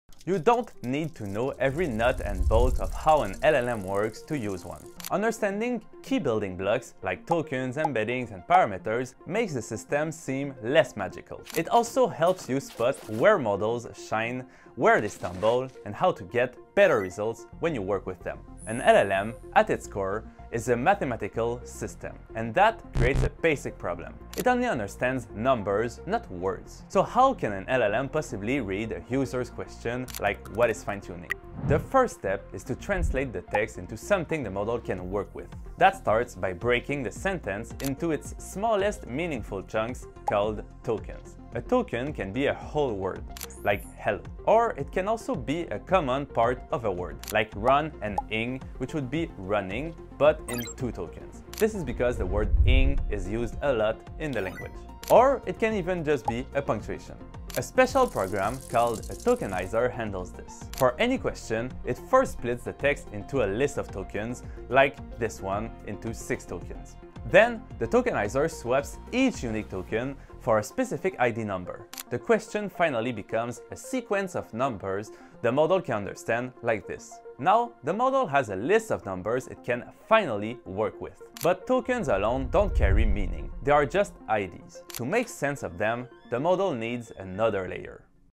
Everything in LLMs starts here
Day 3/42: What is a Token?

Yesterday, we said an LLM is just a powerful autocomplete system.

Today, we define what it’s actually predicting: tokens.

A token isn’t always a word.

It can be part of a word, punctuation, or even a space.

“Running” might become: `runn` + `ing`

And every question you ask is first chopped into these pieces.

Why this matters:

tokens are the currency of LLMs. Cost, speed, context limits, and even failures all start here.

Missed Day 2? That’s where this clicks.

Tomorrow, we’ll see how tokens get meaning instead of just being numbers.